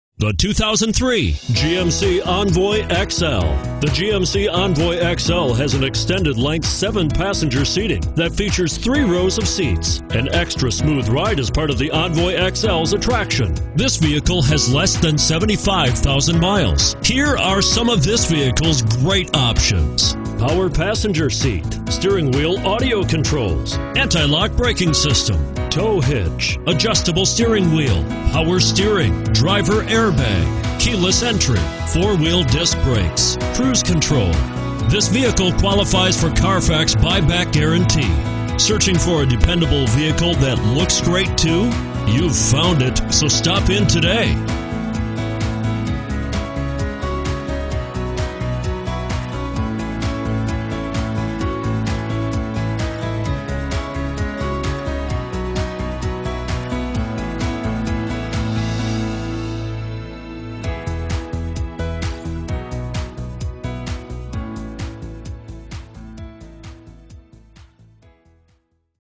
2003 GMC Envoy XL Schererville IN P2552
2003 GMC Envoy XL SLT

Don't miss this 2003 GMC Envoy XL. It's equipped with Automatic transmission and features a Indigo Blue Metallic exterior. With 74420 miles, You'll want to take this car home. Make a great choice today.